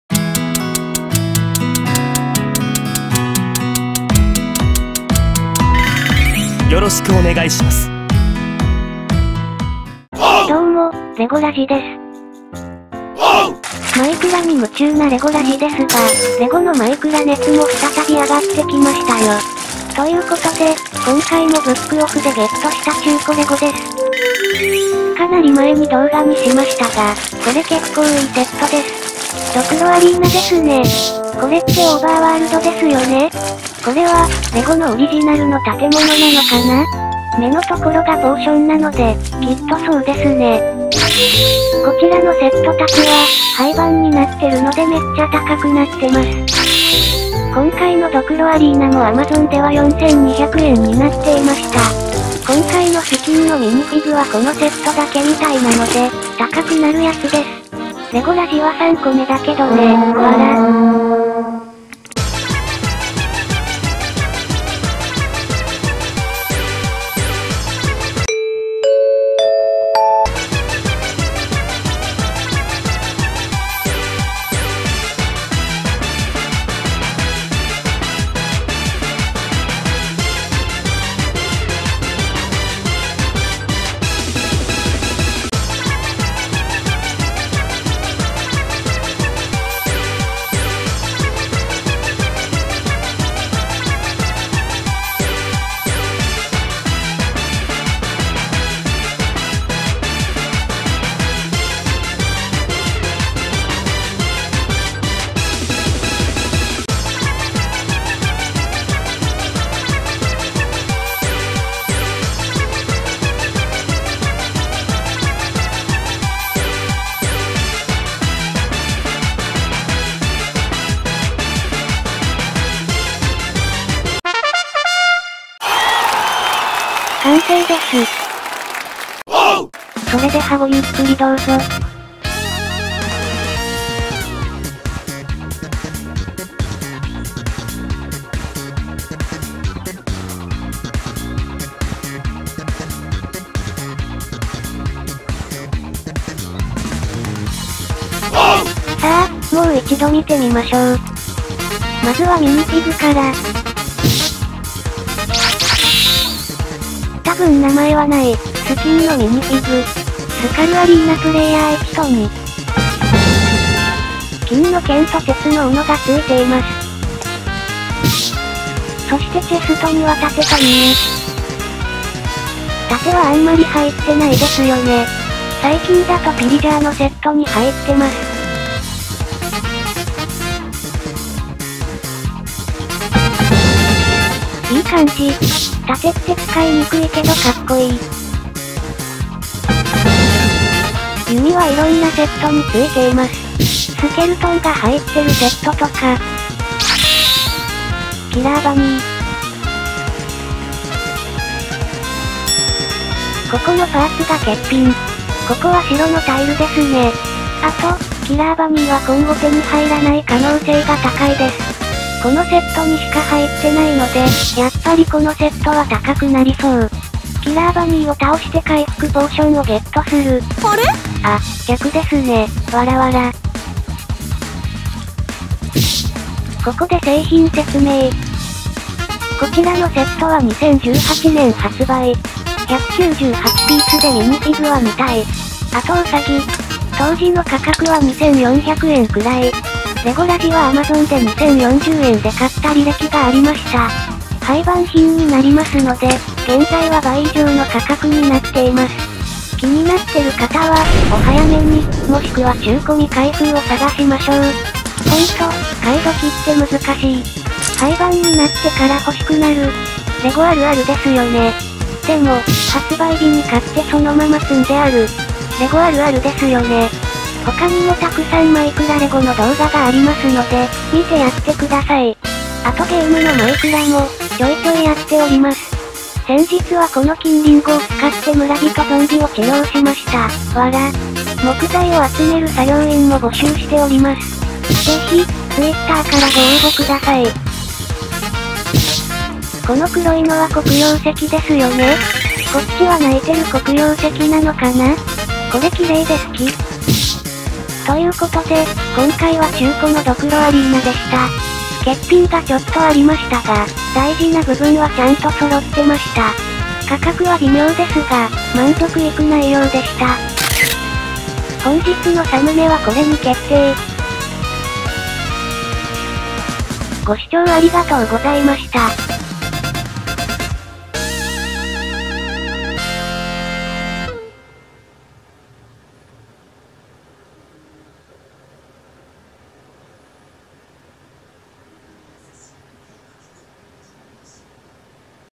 廃盤値上がり中のマイクラレゴ ドクロアリーナ LEGO MINECRAFT 21145 The Skull Arena
キラーバニーにミニフィグ付き。
もう出てこないので、レゴにも出てこないかも？
金のリンゴもゲットできました！

♦Amazonで価格をチェック